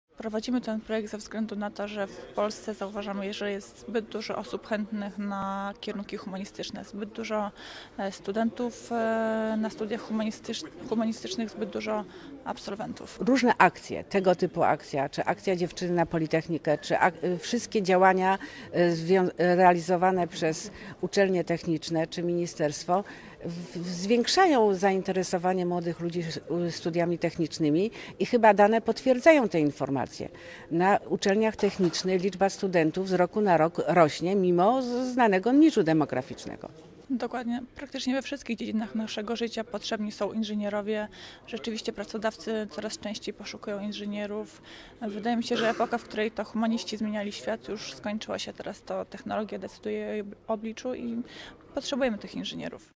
Politechnika Opolska: Promocja kierunków technicznych
Ponad 200 uczniów szkół ponadgimnazjalnych Opolszczyzny i odległego Lubina, wzięło udział w spotkaniu promującym kierunki matematyczne, przyrodnicze i techniczne na Politechnice Opolskiej. Współorganizatorem spotkania było Ministerstwo Nauki i Szkolnictwa Wyższego.